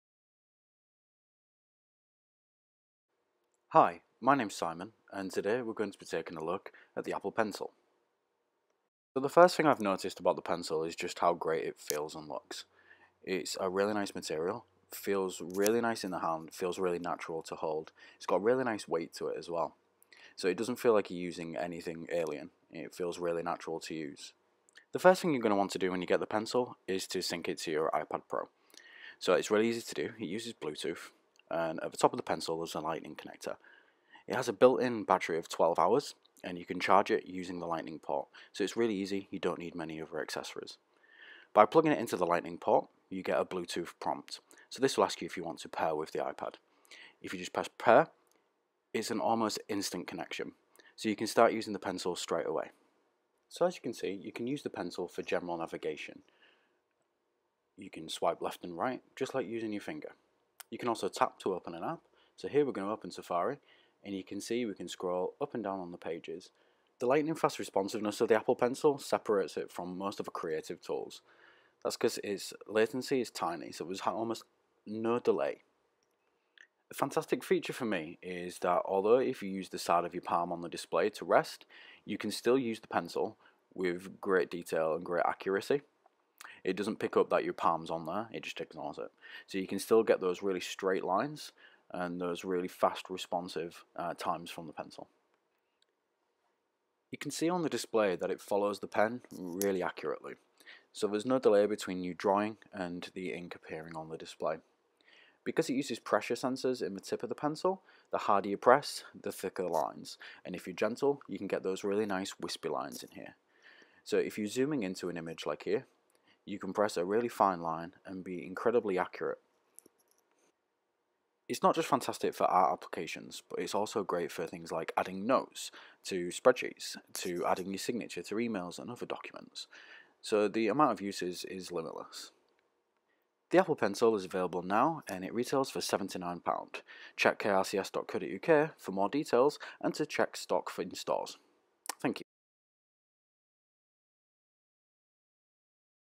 Introducing and setting up the Apple Pencil
Our Hull store team introduce us to the Apple Pencil with a quick guide to getting set up and using Apples latest iPad Pro accessory.